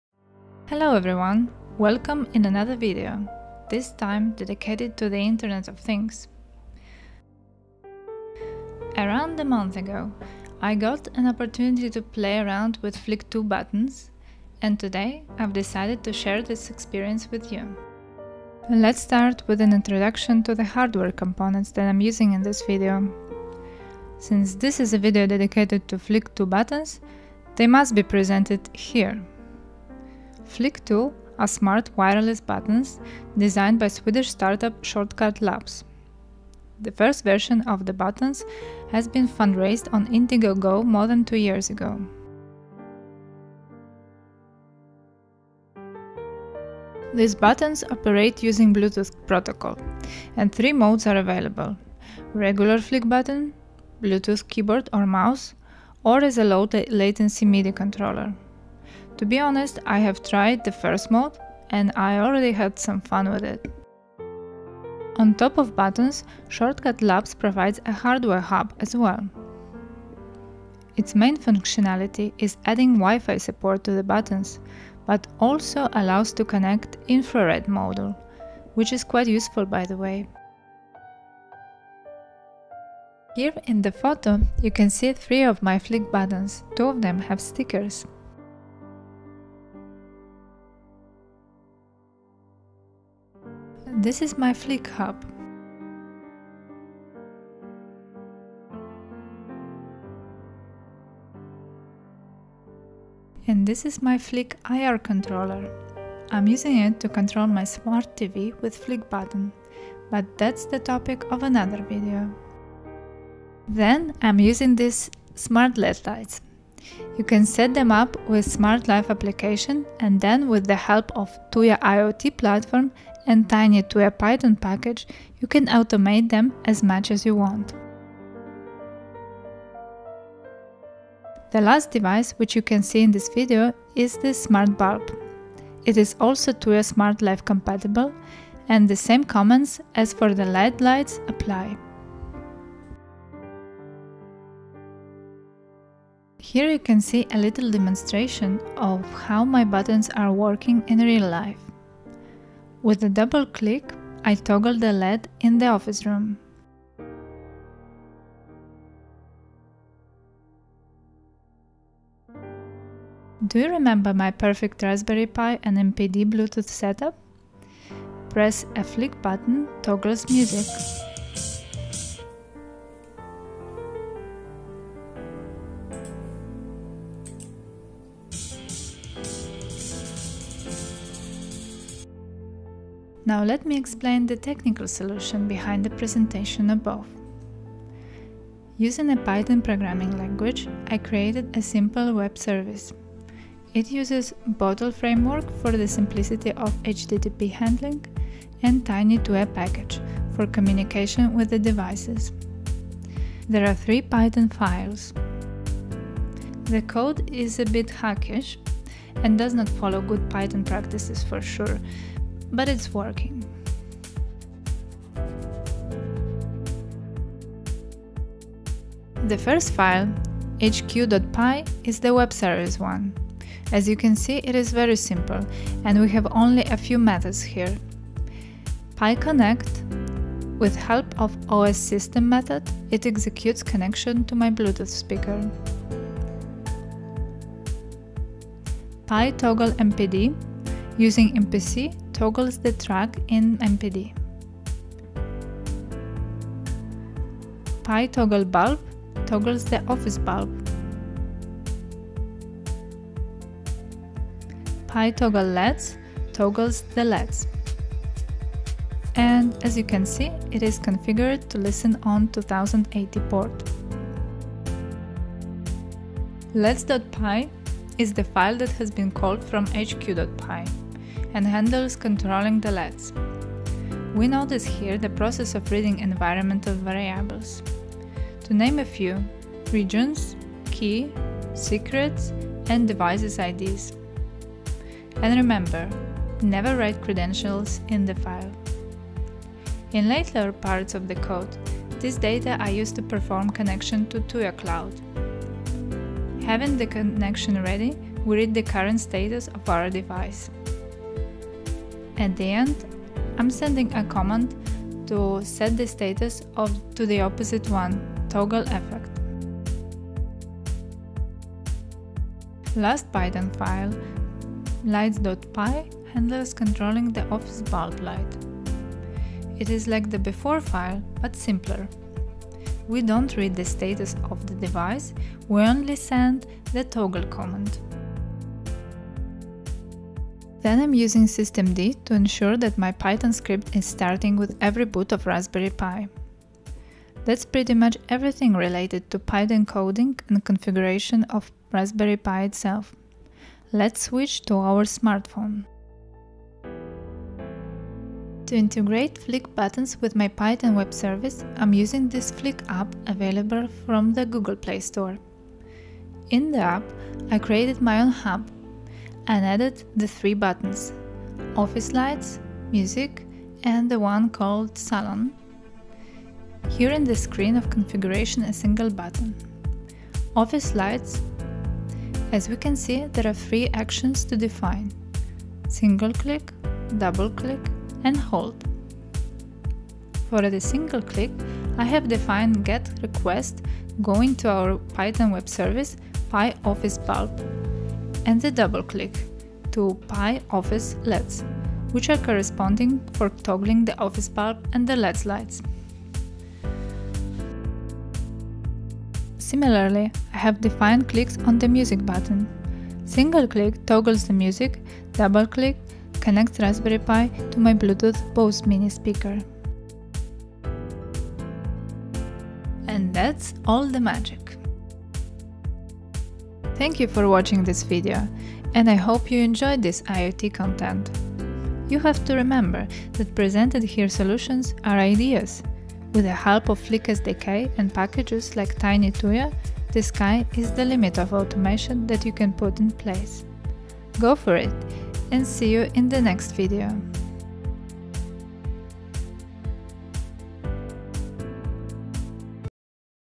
Flic 2 + Raspberry Pi + Python + Tuya everything integrated
Integration of great Flic 2 buttons with Raspberry Pi through some Python code. Controls the music (MPD) and IoT devices (Tuya/SmartLife).

00:00 - Intro
00:18 - What are the Flic buttons?
01:23 - My Setup
02:26 - Demo
03:08 - Code
05:47 - Smartphone Configuration
06:55 - Outro